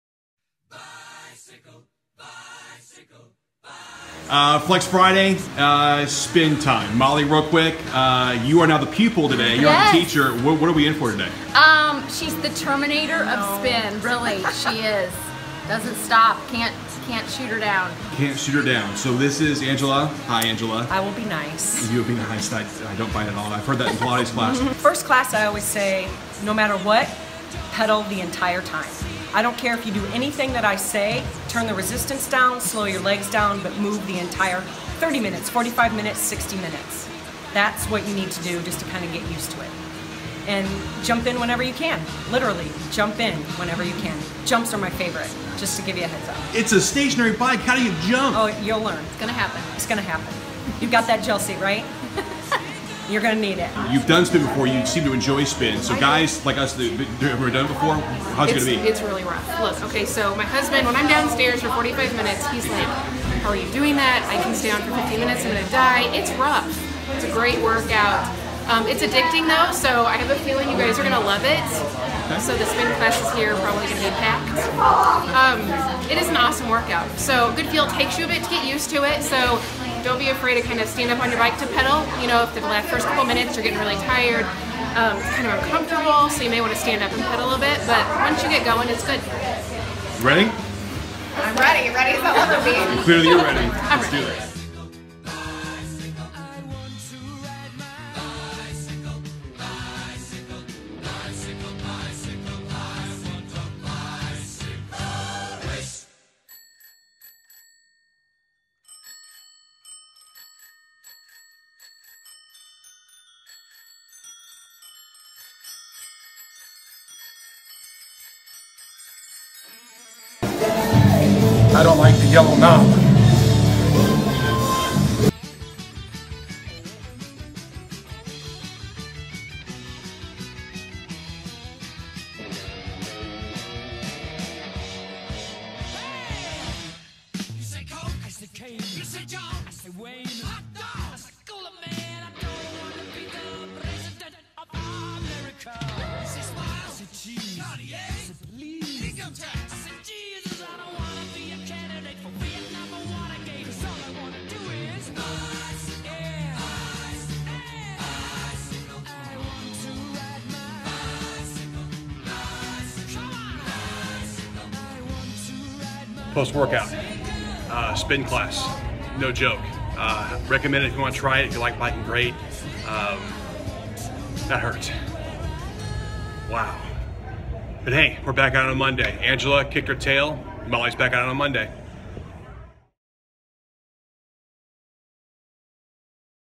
WSOYMCA Challenge Part 2 - Spin
For our next "Flex Friday" class, we ventured into Angela's domain with a spin class. For some of us, it was our first. For others, they were pros!